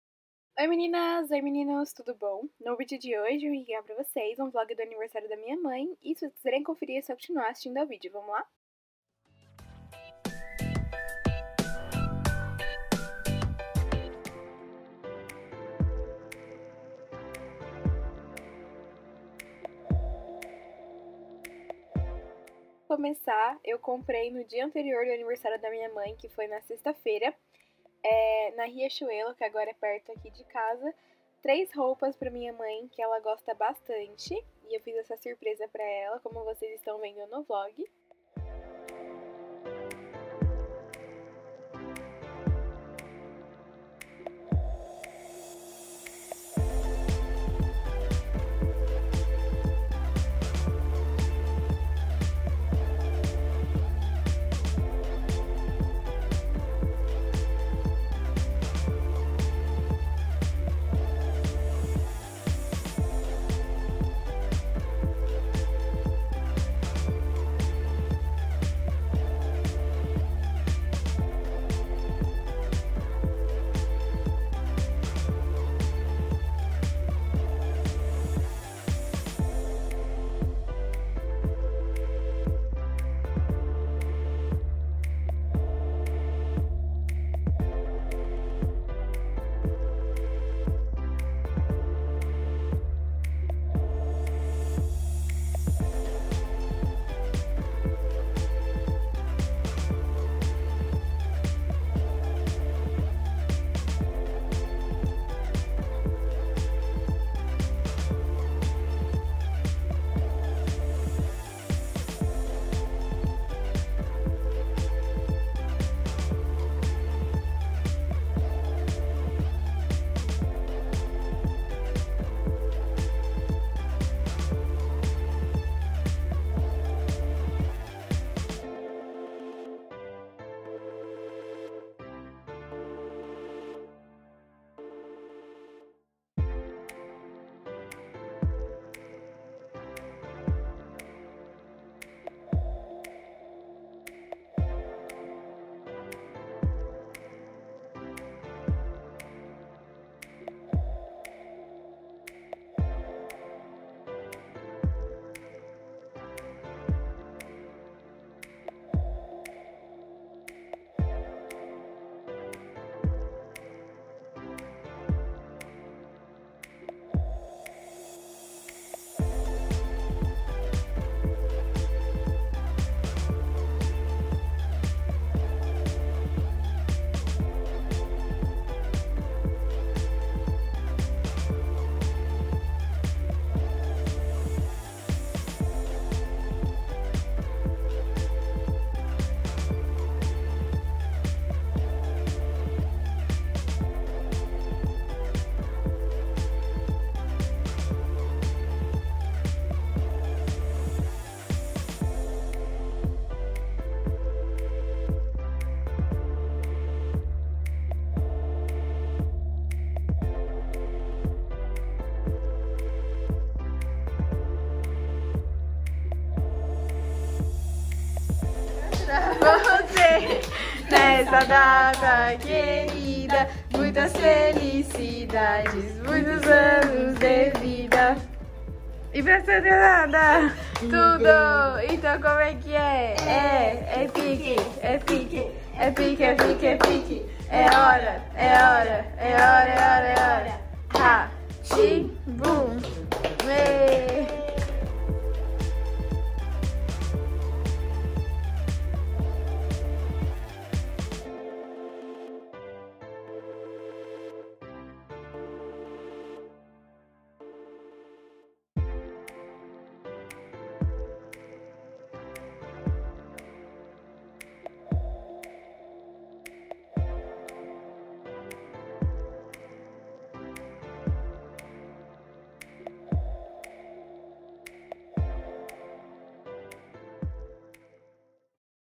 Aniversário da minha mãe - MUITOS PRESENTES!!!
Oi meninas e meninos, tudo bom? Eu sou a Isabela Pasqualin e espero que goste do vídeo.

 Esse video foi sobre o aniversário a minha mãe, foi super simples, mas foi de coração. Teve muita comida, musica, e PRESENTES. Fica essa dica para vocês, mesmo que não saibam o que fazer, peçam um bolinho simples, e faça uma surpresa para ela, que sua mãe ficará MUITO feliz.

Um big big beijão e tchau, tchau!